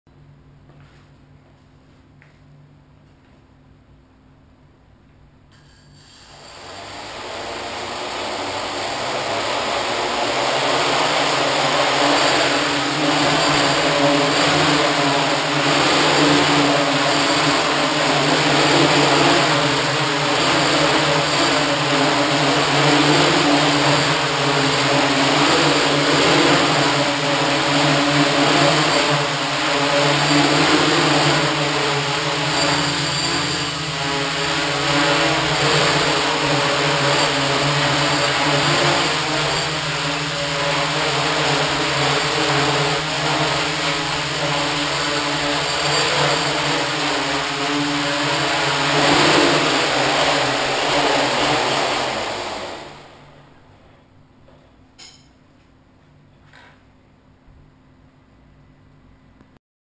AutoQuad 6.6 indoor test
One of my first tests of AQ 6.4 board with AQ 6.6 firmware